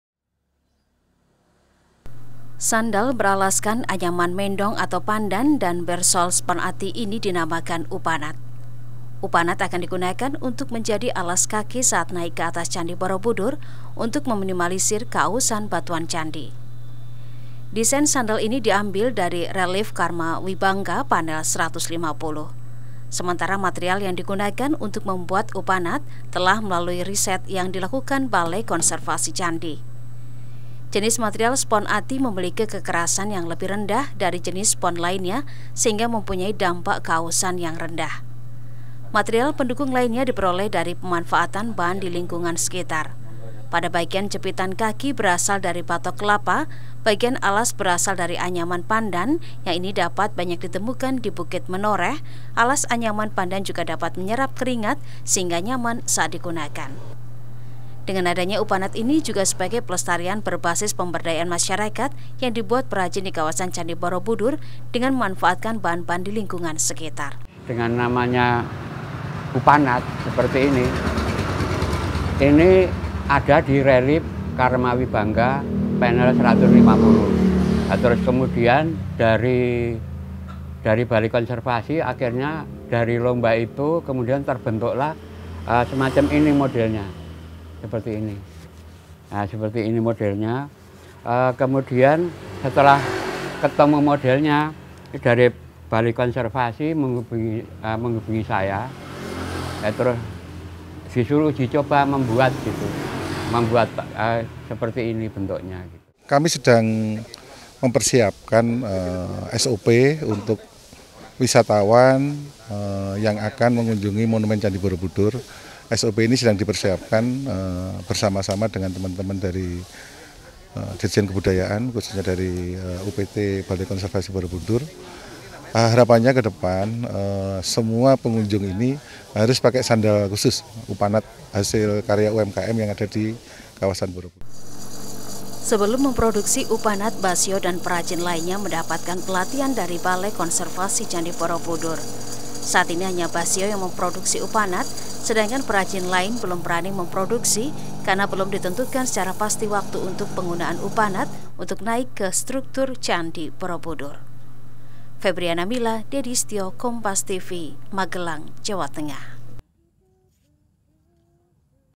Upanat, Sandal Khusus untuk Naik ke Candi Borobudur
MAGELANG, KOMPAS.TV - Upaya meminimalisir keausan batu pada struktur bangunan candi, Balai Konservasi Candi Borobudur mendesain sandal khusus menggunakan material dengan tingkat kekerasan yang rendah.

Sandal beralaskan anyaman mendong atau pandan dan bersol spon ati ini dinamakan upanat. Upanat akan digunakan untuk menjadi alas kaki saat naik ke atas Candi Borobudur untuk meminimalisir keausan batuan candi.

Desain sandal ini diambil dari relief Karmawibhangga panel 150. Sementara, material yang digunakan untuk membuat upanat telah melalui riset yang dilakukan balai konservasi candi.

Jenis material spon ati memiliki kekerasan yang lebih rendah dari jenis spon lainnya, sehingga mempunyai dampak keausan yang rendah.

Material pendukung lainnya diperoleh dari pemanfaatan bahan di lingkungan sekitar. Pada bagian jepitan kaki berasal dari batok kelapa, bagian alas berasal dari anyaman pandan yang ini dapat banyak ditemukan di bukit Menoreh, alas anyaman pandan juga dapat menyerap keringat, sehingga nyaman saat digunakan.

Dengan adanya upanat, ini juga sebagai pelestarian berbasis pemberdayaan masyarakat yang dibuat perajin di Kawasan Candi Borobudur dengan memanfaatkan bahan - bahan dilingkungan sekitar.

"Kami sedang mempersiapkan SOP untuk wisatawan yang akan mengunjungi monumen Candi Borobudur. SOP ini sedang dipersiapkan bersama dengan teman - teman dari dirjen kebudayaan khususnya dari UPT Balai Konservasi Borobudur. Harapannya ke depan semua pengunjung ini harus pakai sandal khusus upanat," kata Pujo Suwarno, WS General Manager TWC.

sebelum memproduksi upanat, basiyo dan perajin lainnya mendapatkan pelatihan dari Balai Konservasi Candi Borobudur. Saat ini hanya Basiyo yang memproduksi upanat, sedangkan perajin lain belum berani memproduksi karena belum ditentukan secara pasti waktu untuk penggunaan upanat untuk naik ke struktur Candi Borobudur.